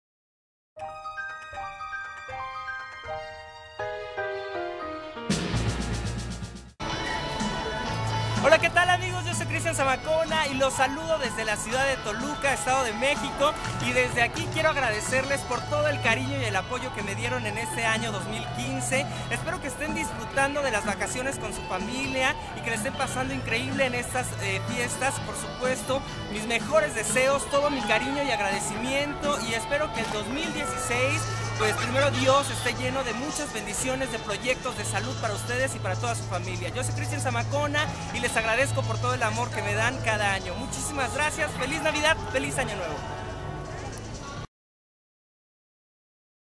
CHRISTIAN ZAMACONA FELIZ NAVIDAD 2015
GRACIAS POR ESTE 2015!!! GRACIAS POR TODO SU CARIÑO MI GENTE, ESTÁN LISTOS PARA RECIBIR EL 2016? FELICES FIESTAS!!! LES DESEO DE TODO CORAZÓN QUE CUMPLAN CADA UNO DE SUS SUEÑOS, BENDICIONES!!!  (VIDEO PUBLICADO ORIGINALMENTE EN LA CUENTA DE FACEBOOK DE CHRISTIAN ZAMACONA) DIC 2015